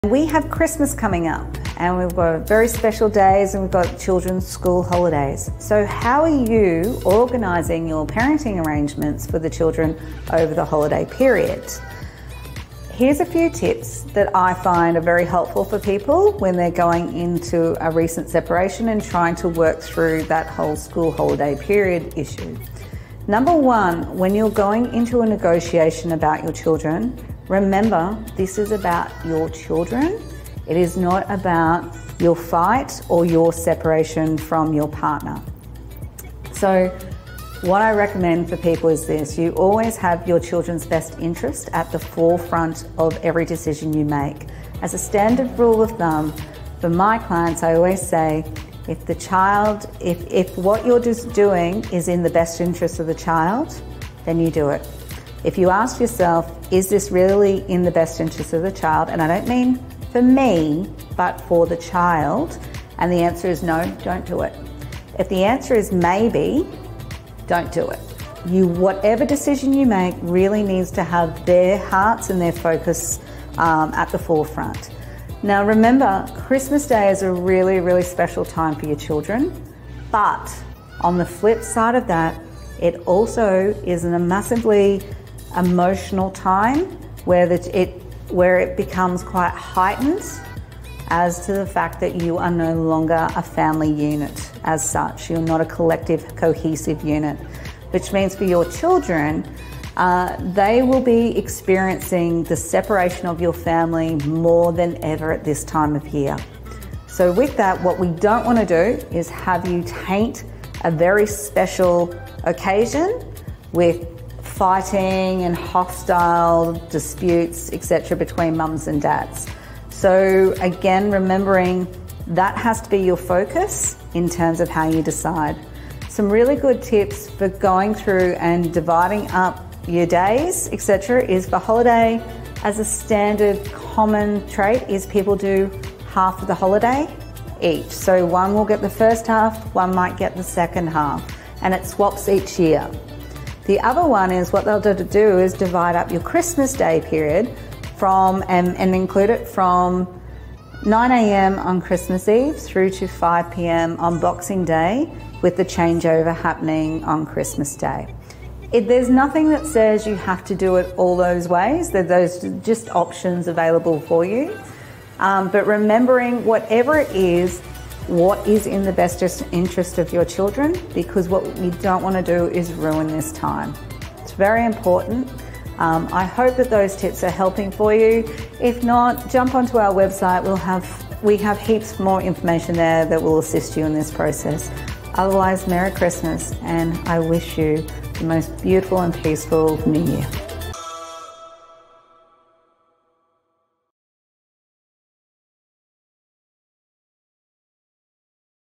Christmas Holidays after Separation - How do I split time with my children over Christmas?
It is that time of year, we have Christmas holidays coming up, we’ve got very special days and we’ve got children’s school Christmas holidays. So, how are you organizing your parenting arrangements for the children over the Christmas holidays?

Perhaps you are recently separated and trying to work through the Christmas holidays challenge for the first time. You may be Divorced and have negotiated your parenting arrangements before. This article will give you a few tips that hopefully will help you find special memories with your children in the Christmas holidays after separation or divorce.